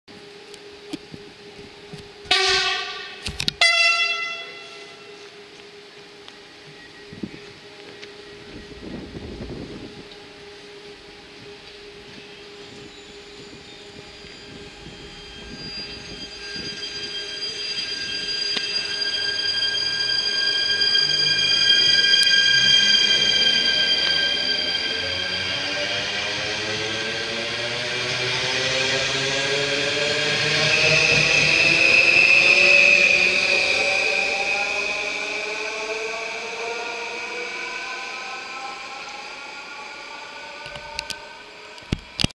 *tyfoon* NS Flirt 2502 vertrekt van station Geldrop
De sprinter (6448) naar Eindhoven